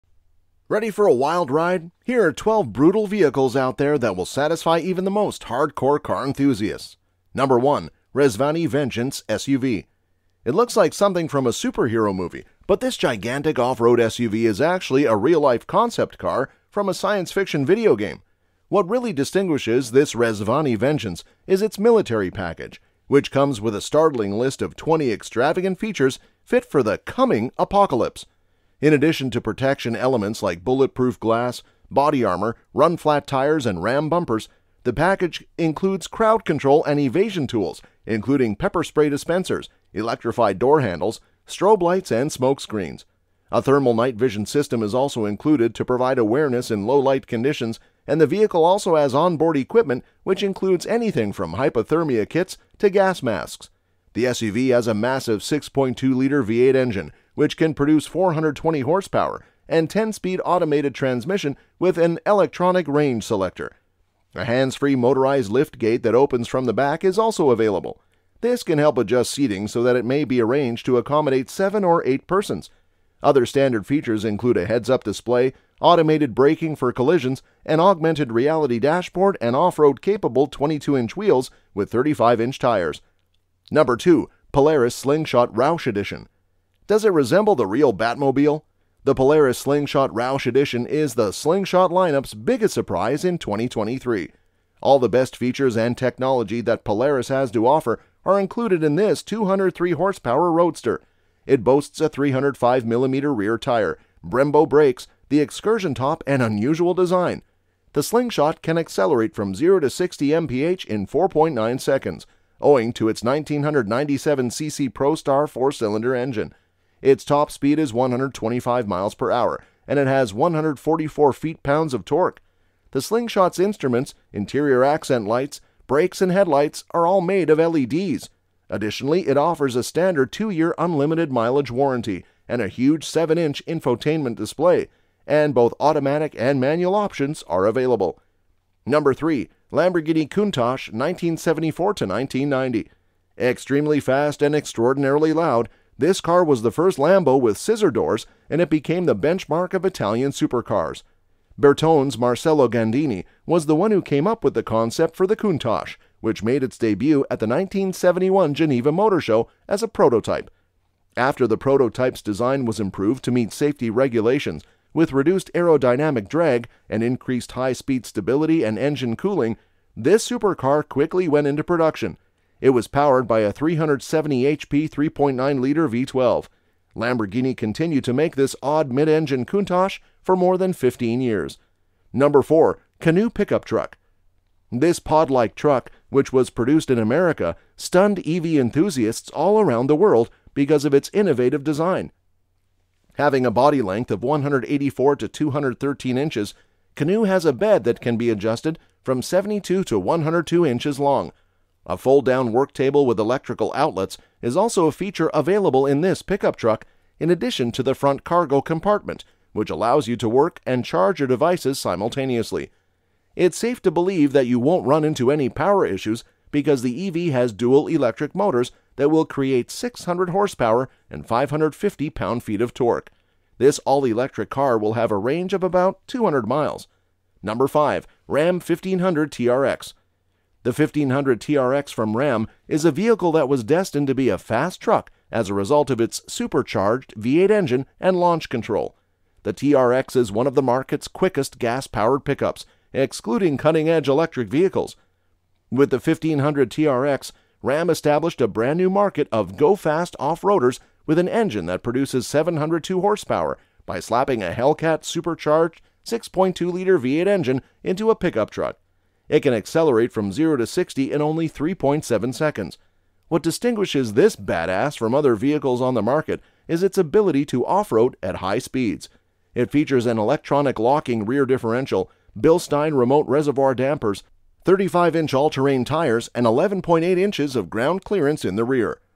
12 Brutal Vehicles That Every Man Will Appreciate
Here are 12 brutal vehicles out there that will satisfy even the most
hardcore car enthusiasts.